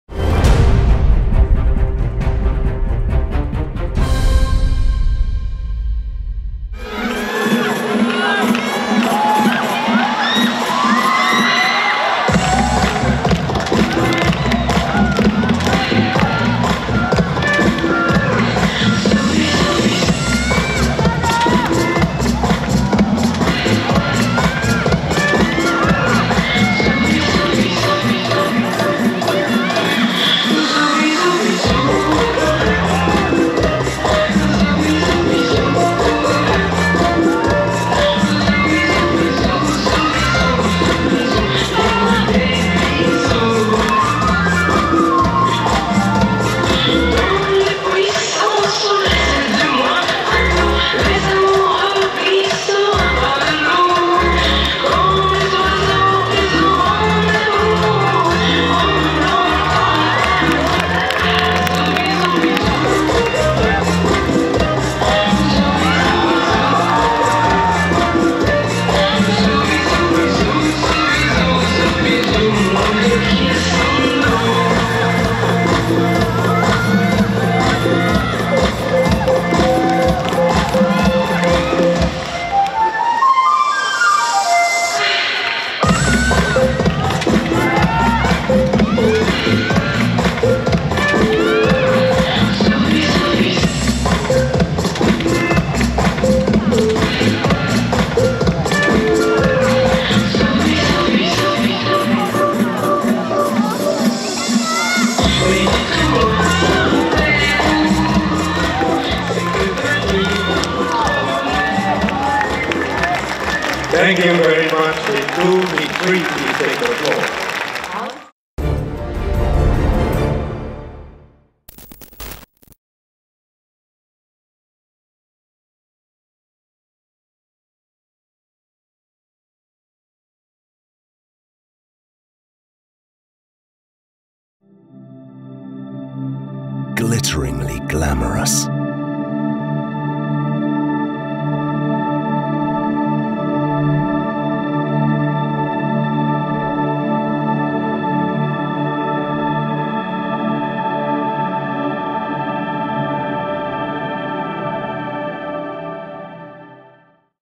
Goffredo - Matus, MDA | 2016 GS LAT Wuhan R2 C | DanceSport Total
Goffredo - Matus, MDA | 2016 GS LAT Wuhan R2 C

Gabriele Goffredo - Anna Matus, MDA, dance the round 2 Cha Cha Cha during the 2016 WDSF GrandSlam Latin Wuhan, CHN, on 16 April.